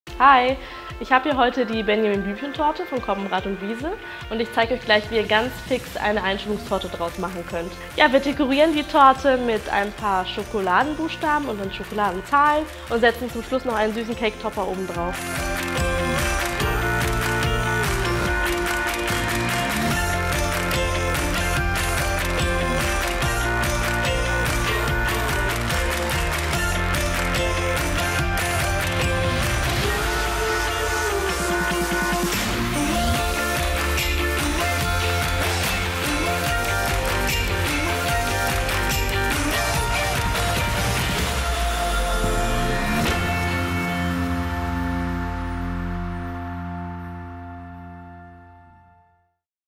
Coppenrath & Wiese – Rezept – Benjamin-Blümchen-Torte zur Einschulung clever pimpen mit Emma
Törööö! Euch fehlt noch eine süße Torten-Idee zur Einschulung? Dann schaut euch unser Video an! Diese Einschulungstorte kann wirklich jeder nach"pimpen"... ;-)